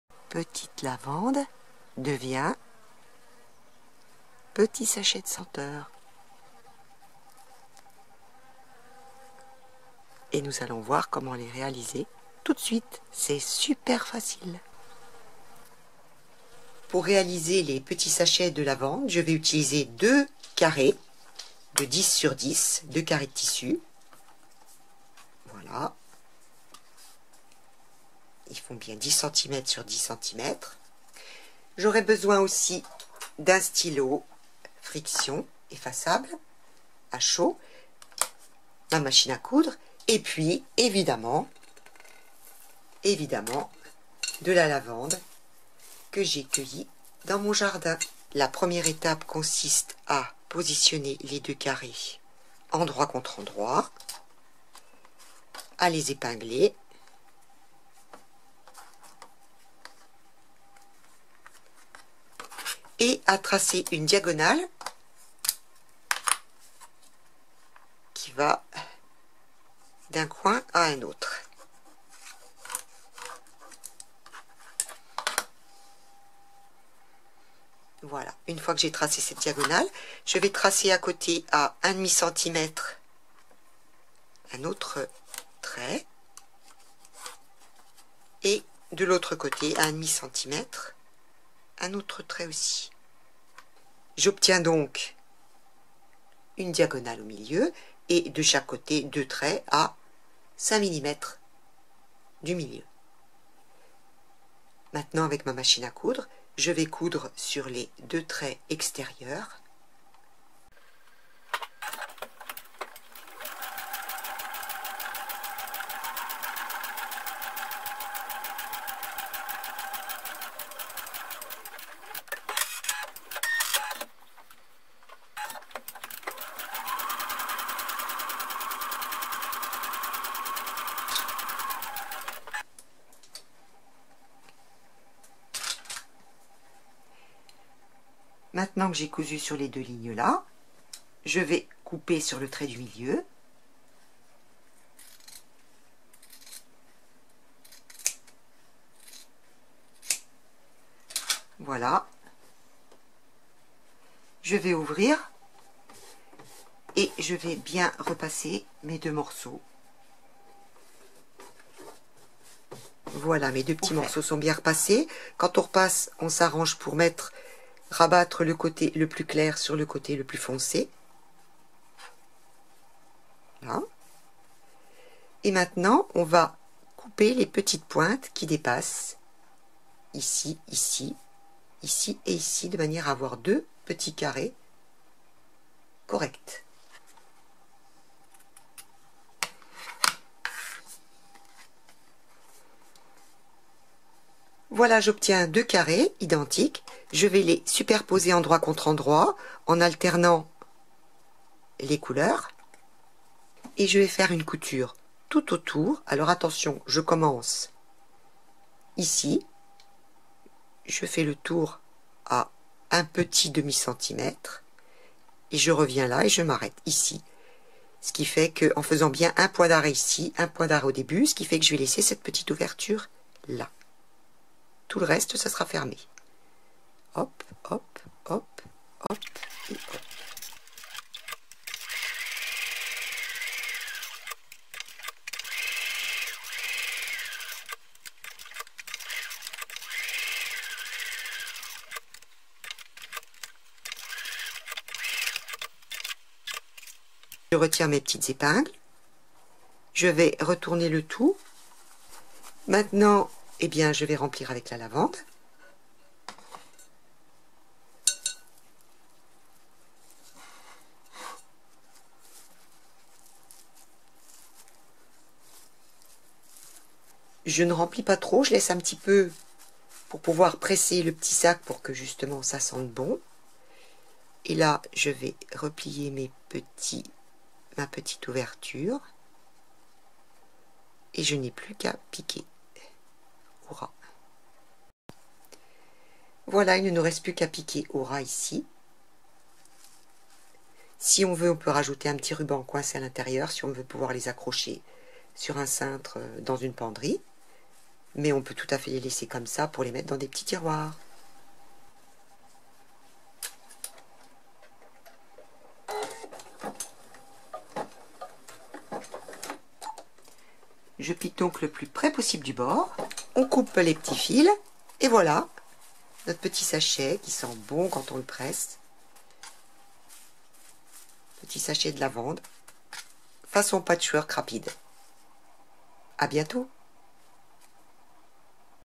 Sachets de lavande : tuto super simple
Un petit tuto super simple pour réaliser des sachets parfumés en utilisant une technique de patchwork; Idéal pour utiliser les chutes !